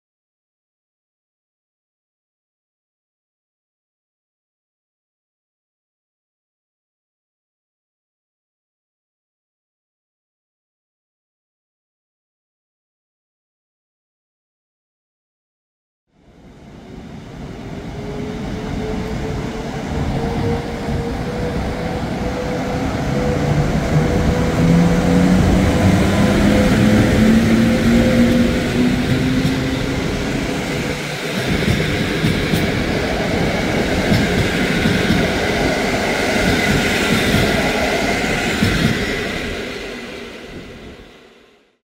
EP09-046 z IC 83106/7 Miechowita do Krakowa Głównego.
EP09-046 od stacji Poznań Główny z IC 83106/7 Miechowita do Krakowa Głównego.

Województwo: Wielkopolskie
Miejscowość: Poznań
Stacja: Poznań Główny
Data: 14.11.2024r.
Godzina: 12:45
Przewoźnik: PKP Intercity